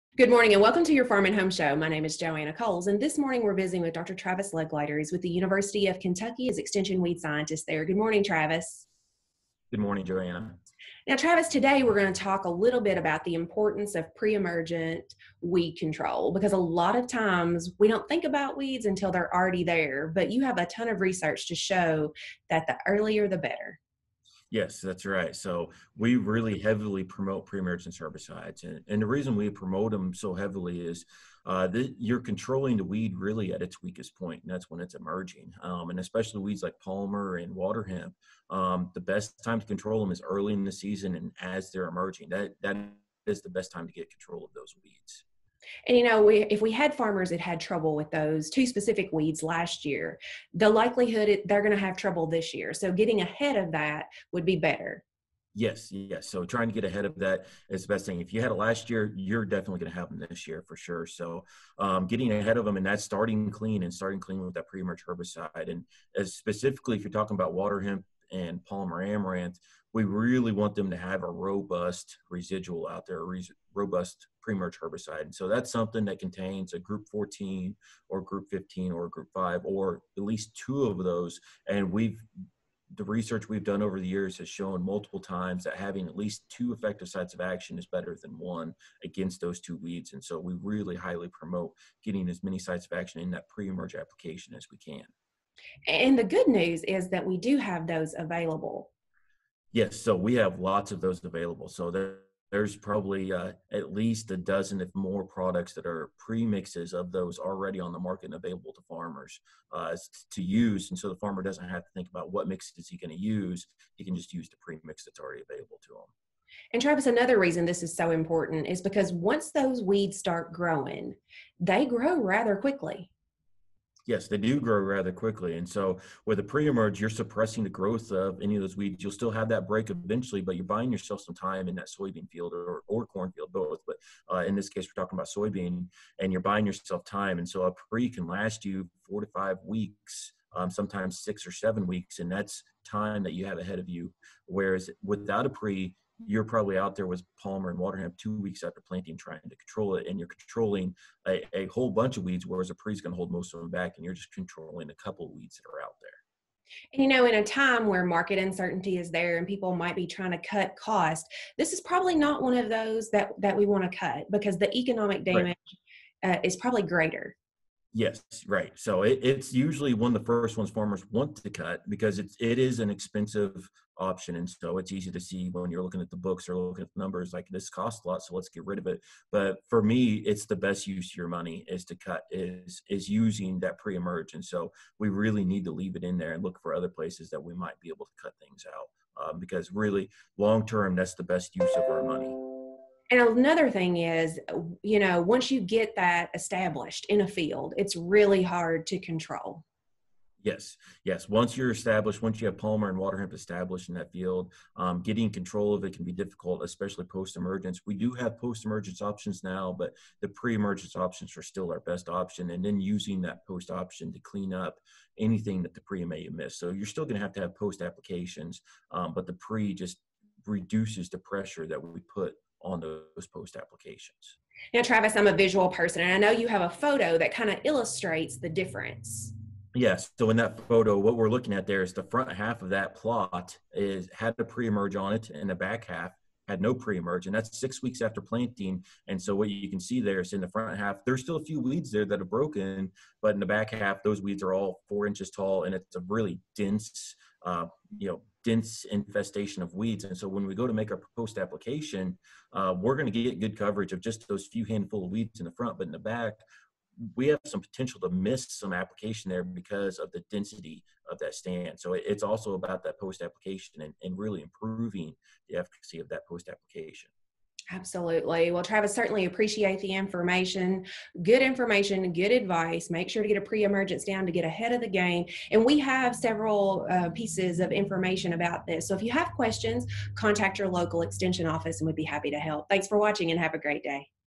Preemergence Herbicides in Soybeans
Joanna visits with Dr. Travis Legleiter, UK Extension Weed Scientist to talk about preemergence herbicides in soybeans.
Weed control in all crops should start at crop planting and is most critical in the early stages of the crop season. The use of preemergence herbicides is the best option farmers have for effective early season weed control, but unfortunately is often the first option to be eliminated from herbicide programs when input cost need to be lowered. The use of preemergence herbicides is crucial for farmers dealing with waterhemp or Palmer amaranth as postemergence options are very limited for these two weeds. Preemergence herbicides control weeds at their most vulnerable stage or emergence and provide suppression of high-density populations allowing for reduced selection pressure on postemergence applications. Resistance to preemergence herbicides is possible, thus the use of multiple sites of action is critical for these products as well, especially when dealing with Palmer amaranth and waterhemp.